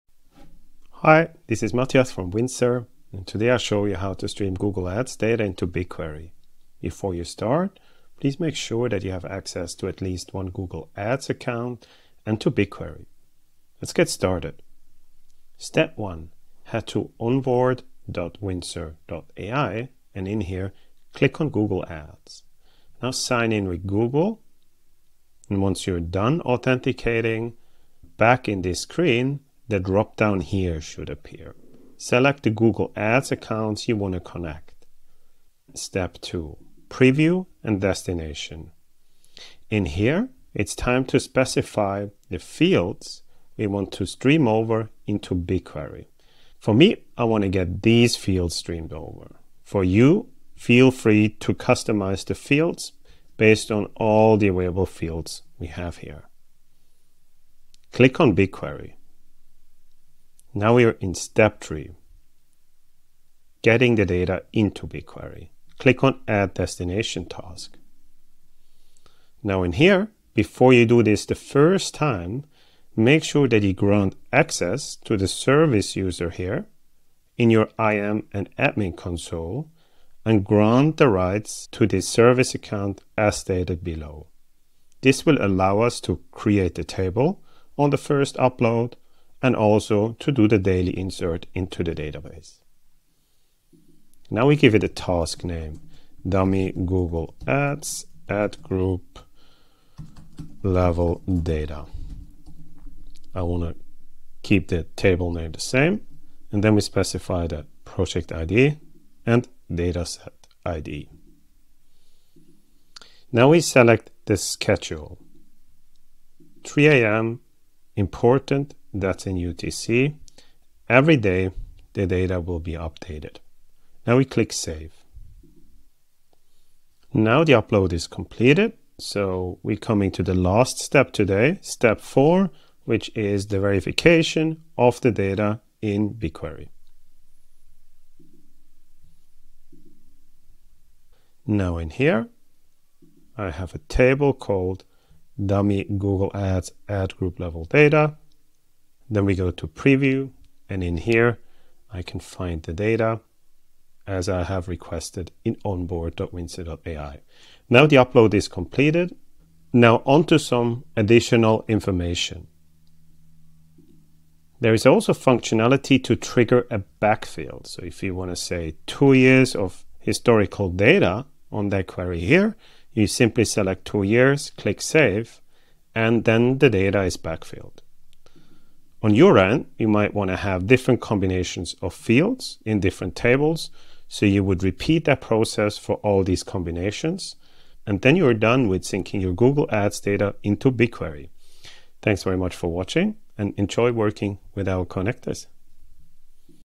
Syncing Google Ads to BigQuery in less than 4 minutes
Learn how to stream Google Ads data into BigQuery in under 4 minutes. Automatically and with no code.

In this tutorial, Matthias shows you how to:
1. Connect your Google Ads account(s) to Windsor.ai
2. Select reporting fields/metrics and preview data
3. Set up the BigQuery destination and schedule a refresh
4. Verify your Google Ads data in BigQuery

Timestamps:
00:00 Intro
00:15 Connecting Google Ads accounts
00:35 Selecting fields and data preview
01:00 Setting up BigQuery data sync
02:11 Checking the data in BigQuery

FAQs:
How do I connect Google Ads to BigQuery?
Answer: Connect Google Ads to Windsor.ai, select fields, choose BigQuery as the destination, then start the sync.

Can I schedule an automatic refresh to BigQuery?
Answer: Yes, you can set a refresh schedule (every 15 minutes, hourly, or daily), so your BigQuery tables stay up to date.

What data levels can I export from the Google Ads API?